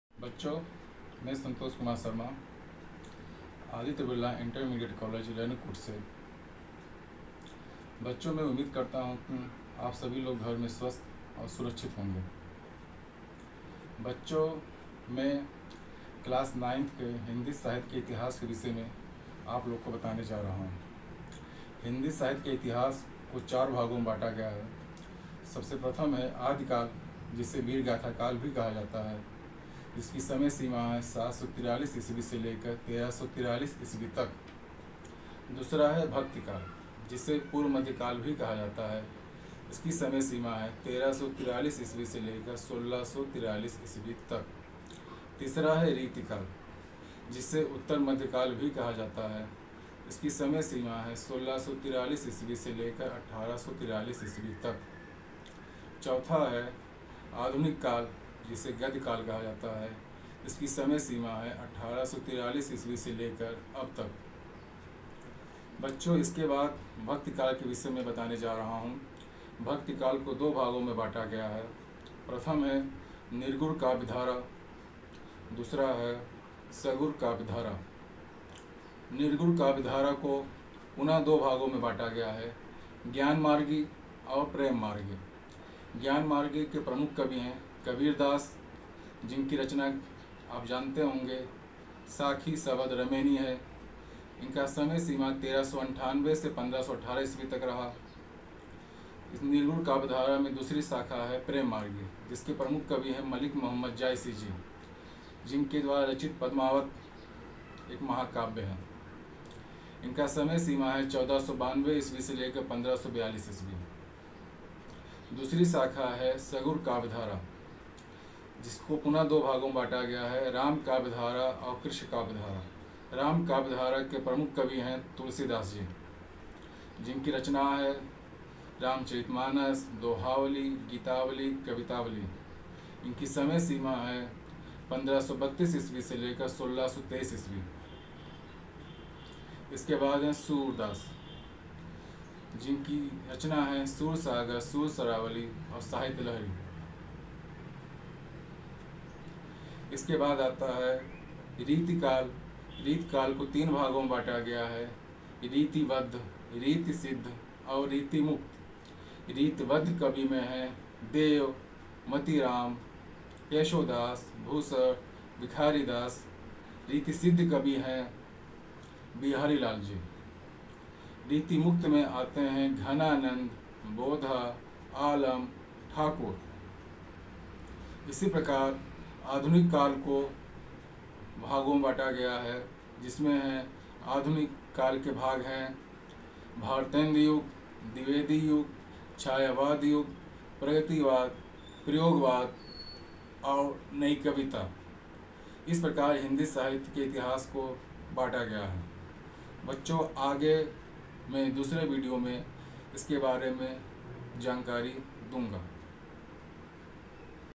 ये विडियो कक्षा 9 एवं 10 दोनों के लिए है शीर्षक -हिन्दी साहित्य का इतिहास, श्री सन्तोष शर्मा
As per UP Board Syllabus